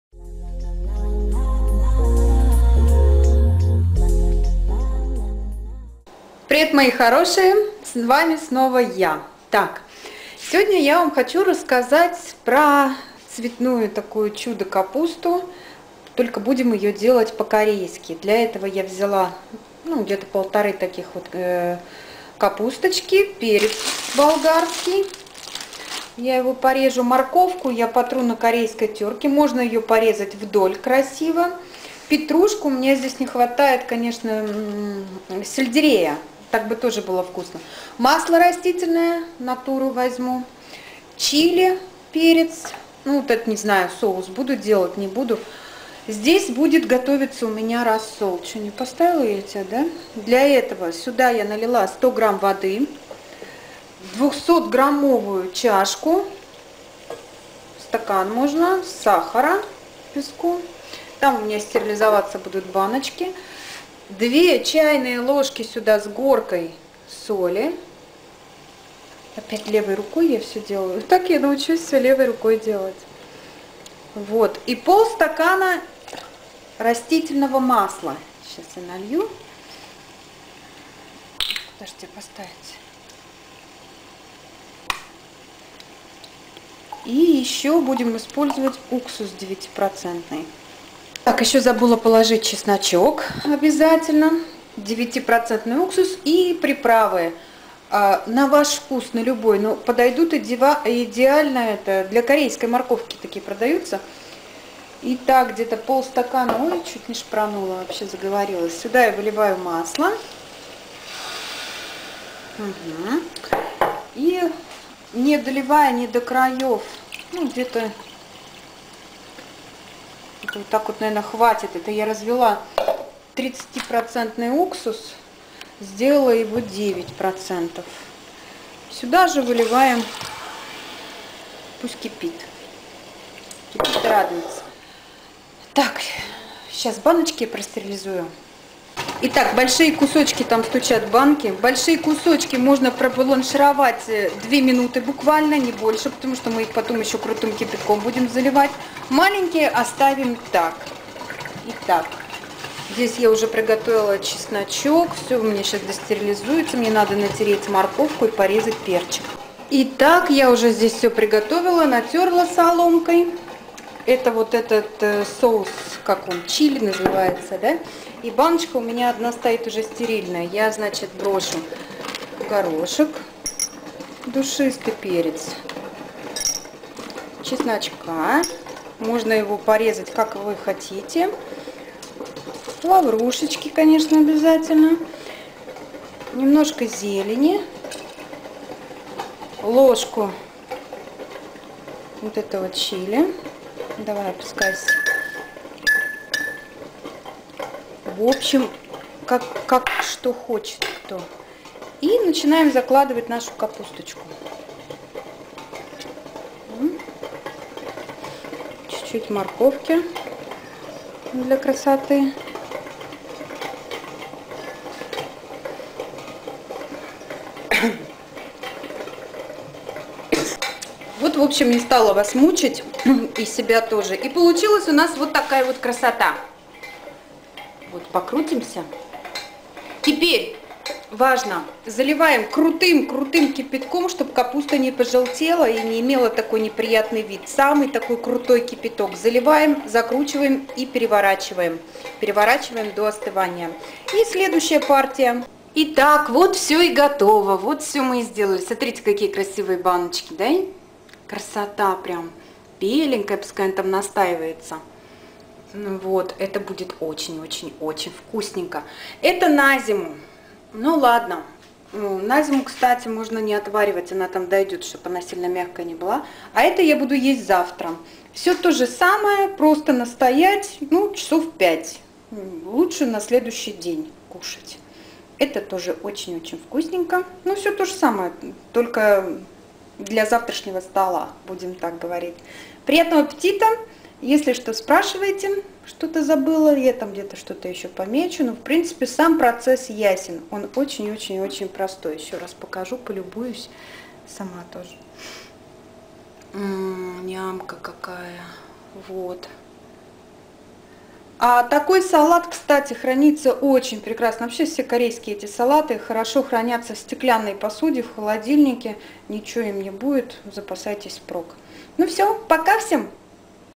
Цветная капуста по - корейски! ОООчень вкусная штука!
Обалденная штука! И так, ловите рецепт: Ингредиенты:
1 средний качан капусты
1 крупная морковь
3-5 зубчиков чеснока(по ходу пьесы.. смотря сколько делать капусты)
2 столовые ложки приправы для моркови по-корейски  (кориандр, паприка, острый красный перец)
1 ПУЧОК ПЕТРУШКИ
стебли сельдерея по желанию (для варианта закатывания на зиму!)
пару капель Табаско
жгучий перец (по вкусу)
Маринад:
1 ЛИТР воды( в видео оговарилась 100гр,ну куда там 100грамм то, 100 гр потом уже под капусту)
1 стакан сахара (Стаканы используем 200 граммовые)
0,8 стакана уксуса 9%
0,5 стакана растительного масла
2 СТОЛОВЫЕ ложки с горкой соли
Капусту разобрать на маленькие(ай, да на любые) соцветия. Добавить натертую соломкой морковь, разрезанный пополам чеснок, приправы и перемешать:
Маринад прокипятить пару минут и сразу залить им овощи:Перед подачей настоять сутки при комнатной температуре: